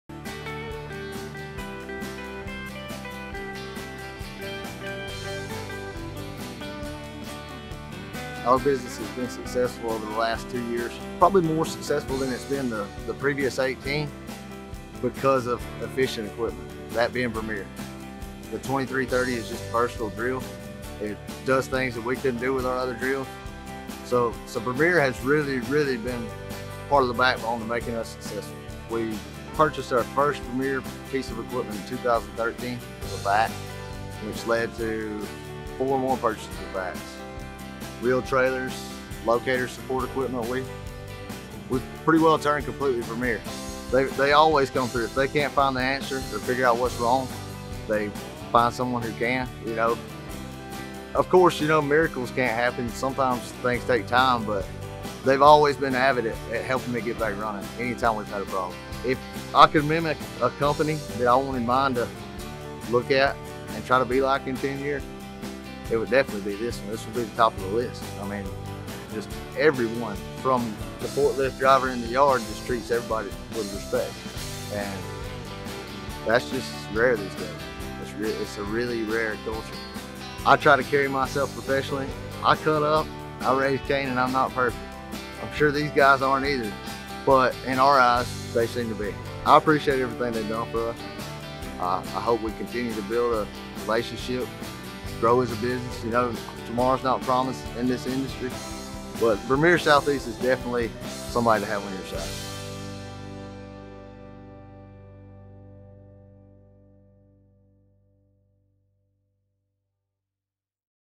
Odie Bore • Tallapoosa, GA
One of Vermeer Southeast's valued customer partners, Odie Bore, based in Tallapoosa, Georgia, is primarily a power contractor for primary and service repair. They also provide replacement pipe installation on wind farms for fiber control lines and 1250 lines feeding from turbines to sub-stations. They operate various Vermeer equipment, including multiple directional drills and vacuum excavators, as well as DCI locators and Yanmar compact excavators. Listen as John Odom of Odie Bore relates his experience as a Vermeer Southeast partner.